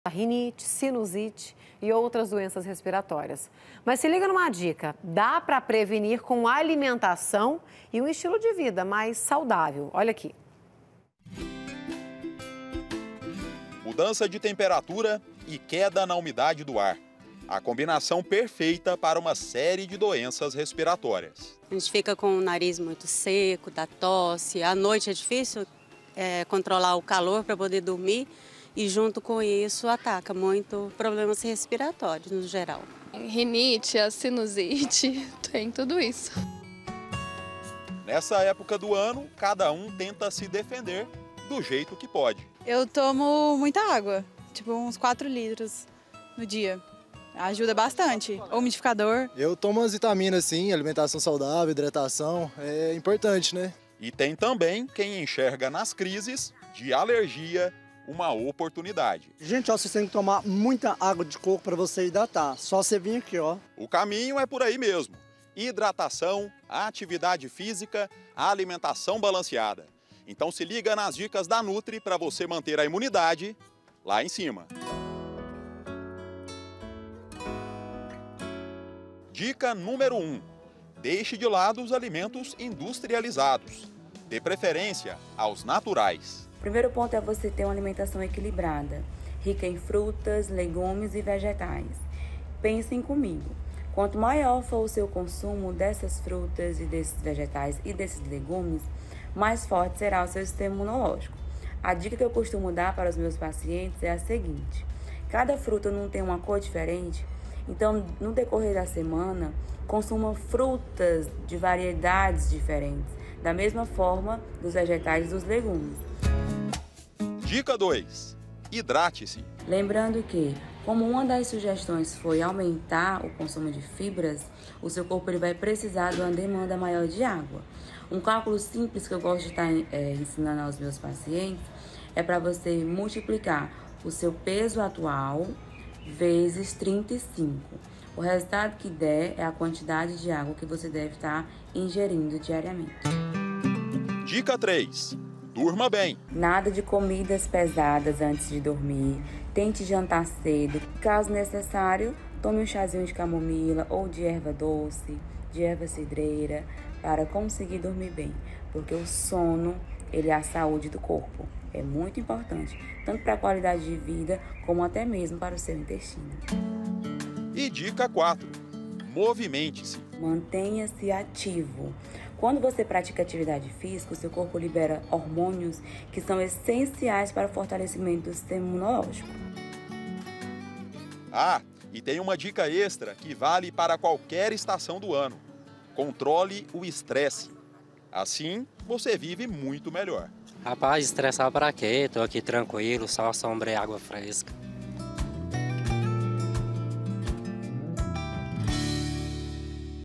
DICAS DE SAÚDE: COMO PREVENIR AS DOENÇAS QUE CHEGAM COM O OUTONO
O OUTONO CHEGOU E TROUXE COM ELE DOENÇAS QUE SÃO COMUNS DESSA ÉPOCA DO ANO POR CONTA DO CLIMA. É O CASO DA RINITE, SINUSITE E OUTRAS DOENÇAS RESPIRATÓRIAS. MAS SE LIGA NESSA DICA: DÁ PRA PREVENIR COM ALIMENTAÇÃO E UM ESTILO DE VIDA MAIS SAUDÁVEL!